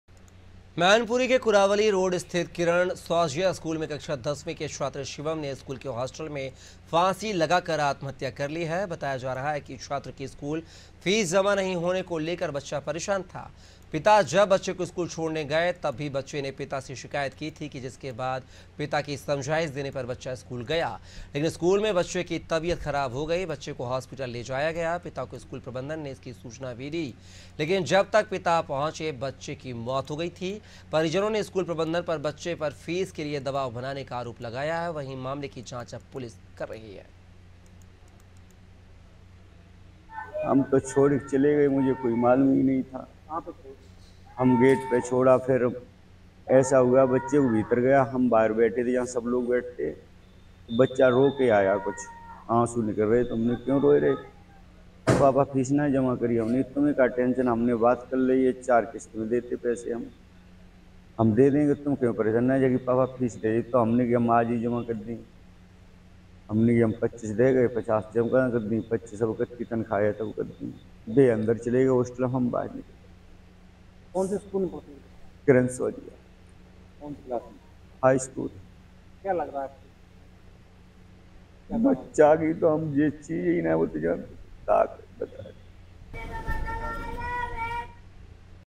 Mainpuri :स्कूल में हुई बच्चे की मौत, स्कूल प्रबंधन पर लगे आरोप | Hindi News || Anaadi Tv
AIRTEL - 379